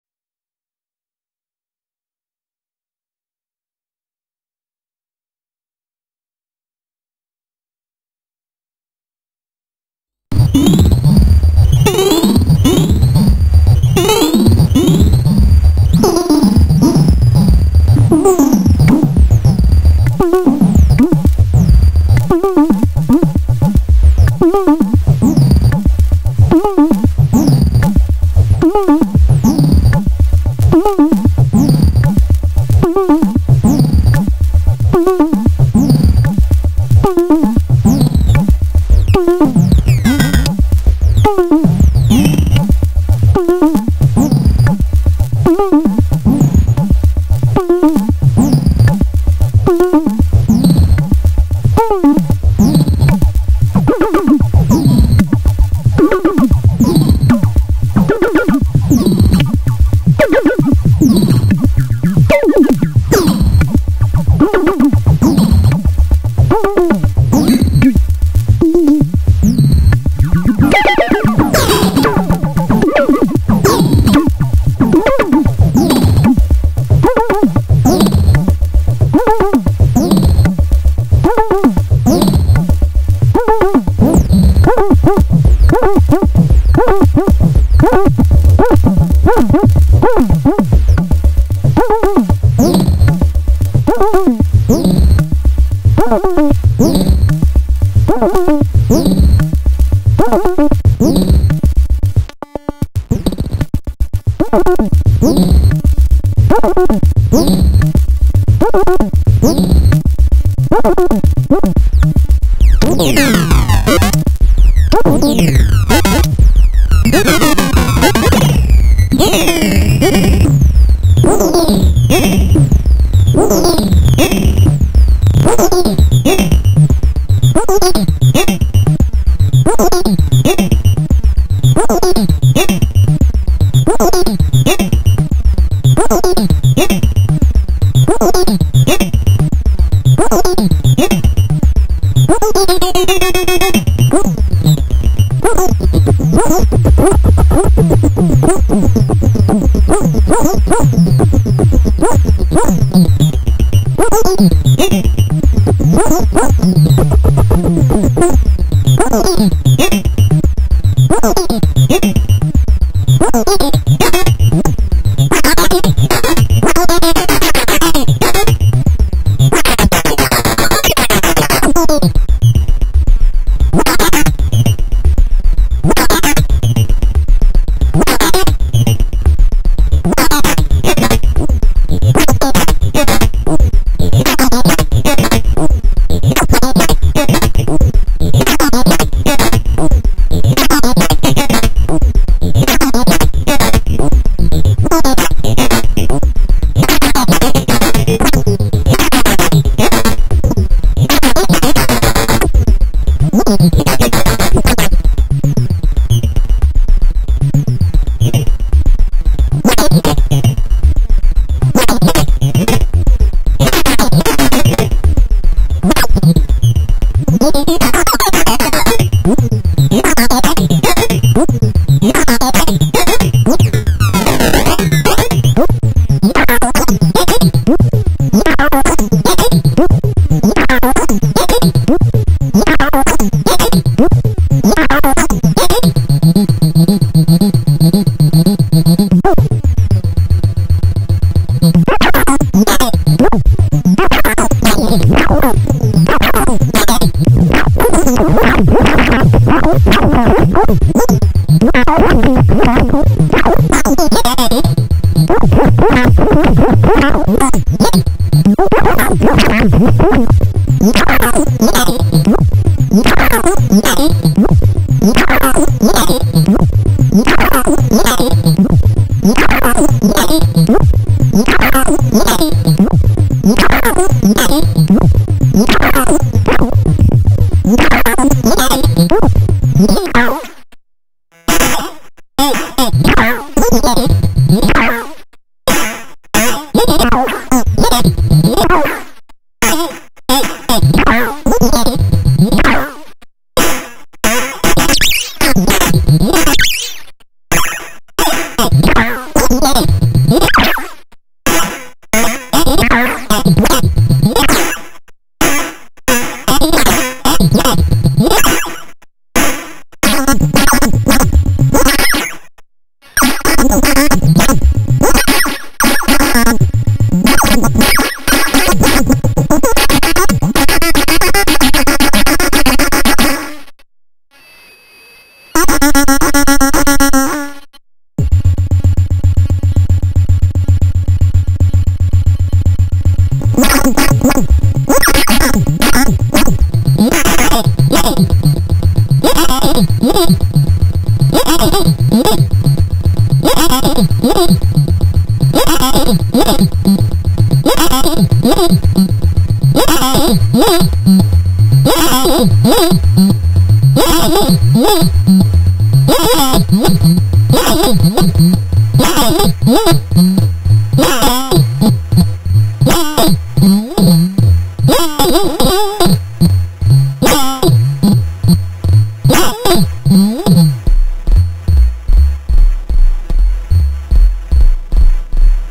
RS110 & Plague bearer
The AS RS110 & Plague bearer tickling each other, featuring on random: Thonk! turing sequencer. Processing through the A-189-1 in delay mode.